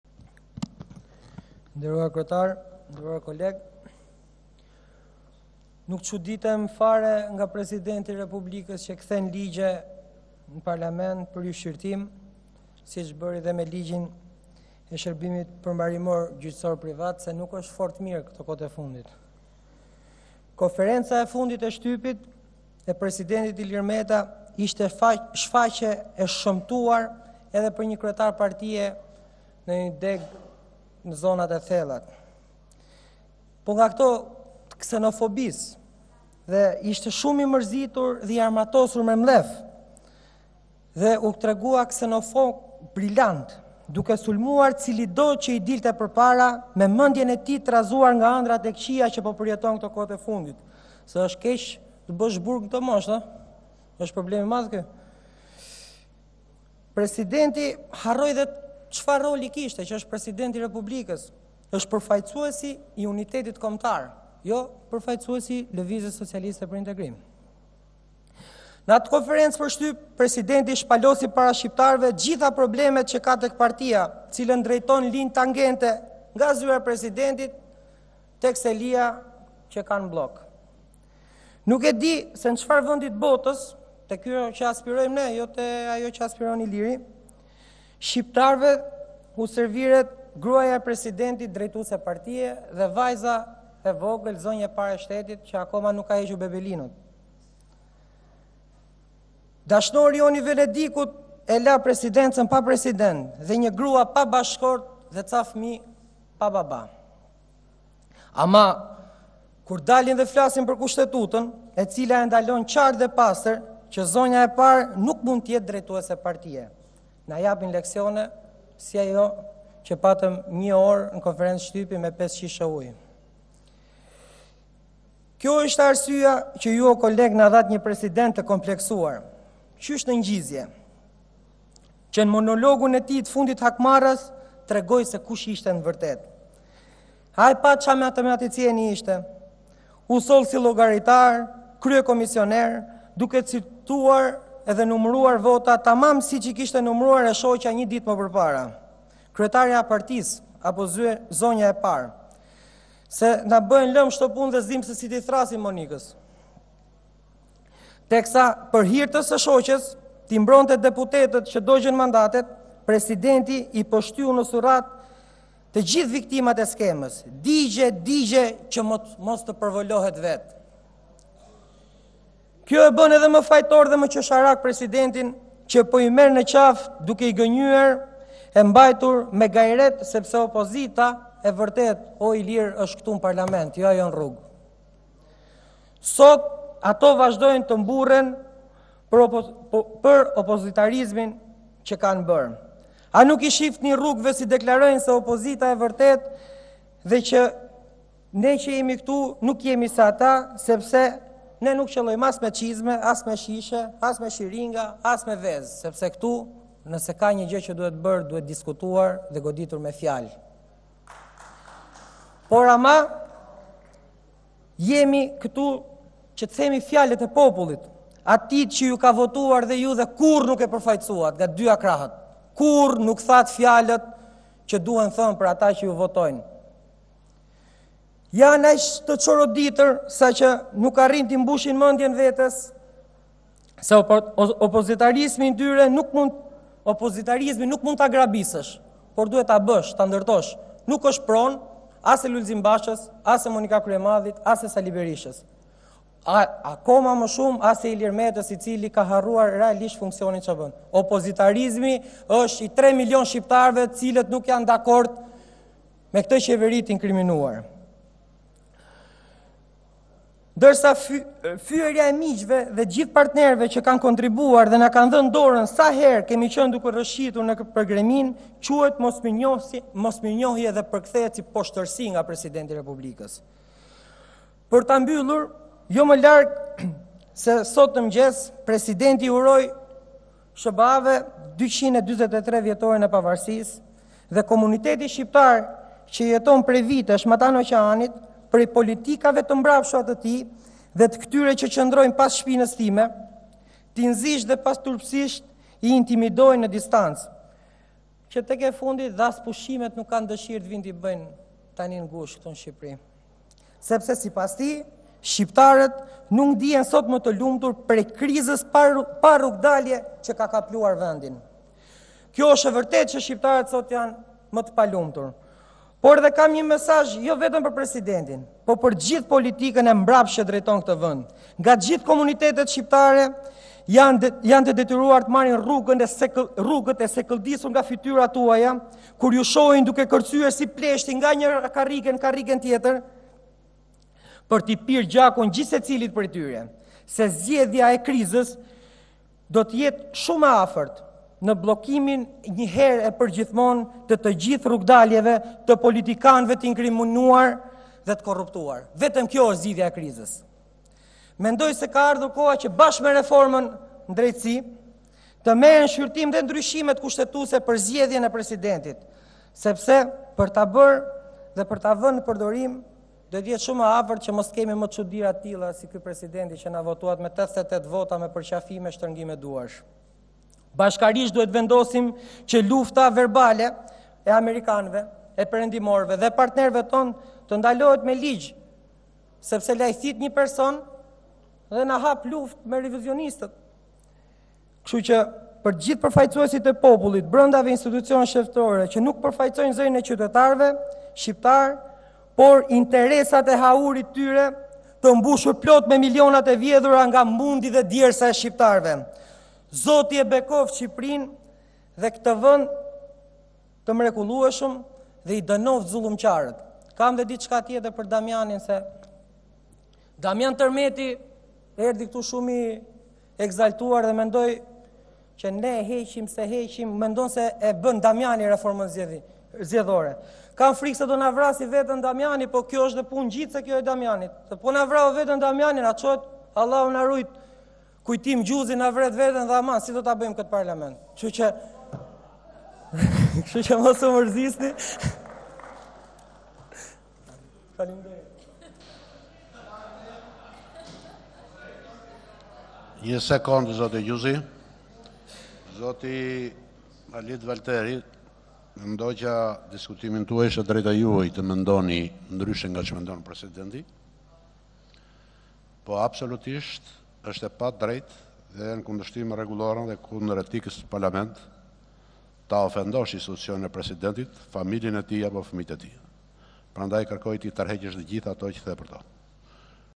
Deputeti demokrat Valteri 'ofendon' presidentin, Ruçi: Tërhiqi të gjitha ato që the për Metën
Deputeti demokrat Halit Valteri gjatë fjalës së tij në Kuvend sulmoi Presidentin Meta dhe familjen e tij pas konferencës së fundit ku kërkoi mes të tjerash tre zgjedhje në Tetor, të përgjithshme, lokale dhe për Presidentin.  "Nuk çuditem me Presidentin, që kthen ligje, pasi nuk është fort mirë këto kohët e fundit. Konferenca e fundit e Presidentit ishte shfaqje e shëmtuar.

Ishte shumë i mërzitur dhe armatosur me mllef, sulmoi cilindo që i dilte përpara me mendjen e trazuar nga ëndrrat, se është keq të bësh burg në këtë moshë, është problem i madh. Presidenti harroi edhe rolin që kishte, përfaqësuesi i unitetit kombëtar dhe jo i LSI-së.

Ai shpalosi problemet që ka te partia në Bllok, nuk e di se cfarë vendi të botës, tek kjo që aspirojmë ne dhe jo ajo që  aspiron Ilir, shqiptarëve u serviret gruaja e Presidentit si drejtuese partie dhe vajza e vogël, zonjë e parë e shtetit, që akoma nuk ka hequr bebelinën" tha ai.

Kryetari i Kuvendit, Gramoz Ruçi ndërhyri duke i kërkuar që të tërheqë ofendimet që lëshoi në drejtim të Presidentit dhe familjes së tij.  "Zoti Valteri është e padrejtë dhe kundër etikës ta ofendosh Presidentin dhe familjen e fëmijët e  tij, prandaj kërkoj ti tërheqësh ato që the për atë" i kërkoi Ruçi.